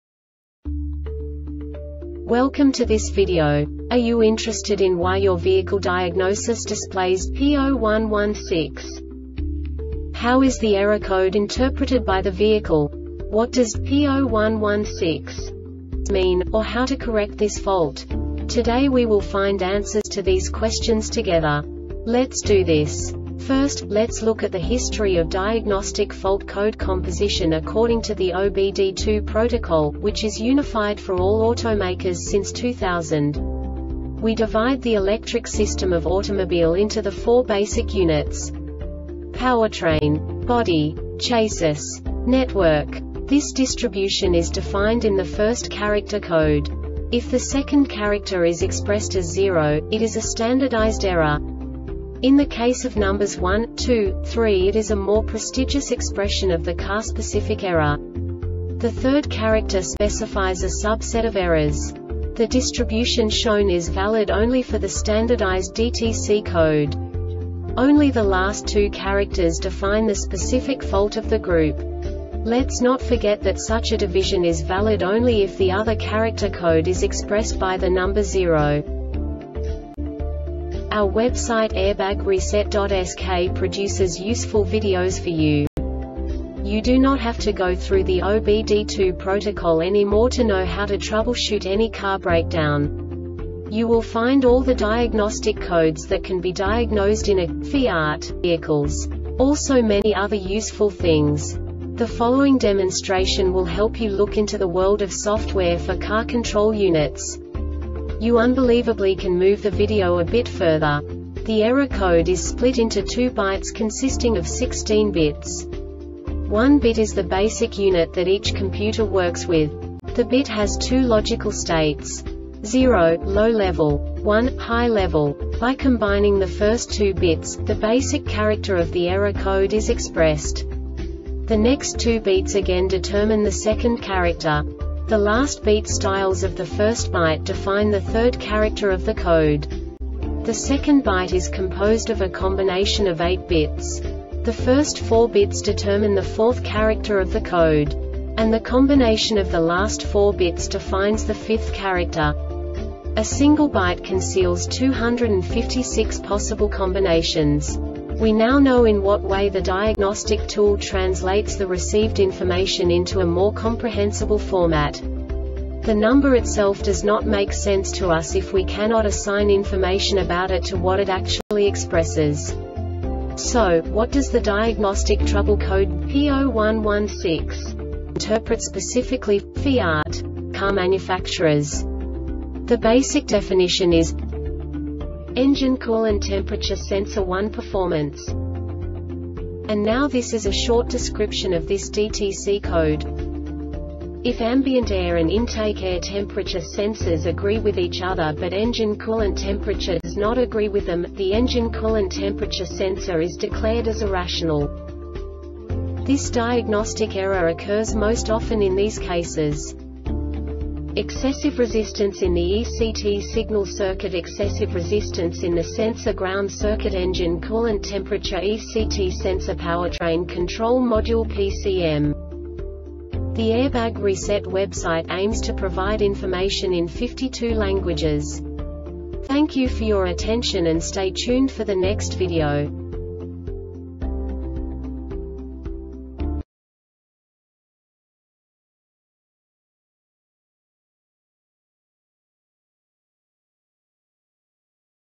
DTC Fiat P0116 Short Explanation
Contents:
0:21 Basic DTC analysis according to OBD2 protocol standard.
1:48 Insight into programming
2:58 A diagnostically understandable description of the Diagnostic Trouble Code
3:05 Basic error definition

Make:
FIAT
Code:
P0116
Definition:
Engine Coolant Temperature Sensor 1 Performance
Description:
If Ambient Air and Intake Air Temperature Sensors agree with each other but Engine Coolant Temperature does not agree with them, the Engine Coolant Temperature Sensor is declared as irrational.
Cause:
Excessive Resistance In The Ect Signal Circuit
Excessive Resistance In The Sensor Ground Circuit
Engine Coolant Temperature (Ect) Sensor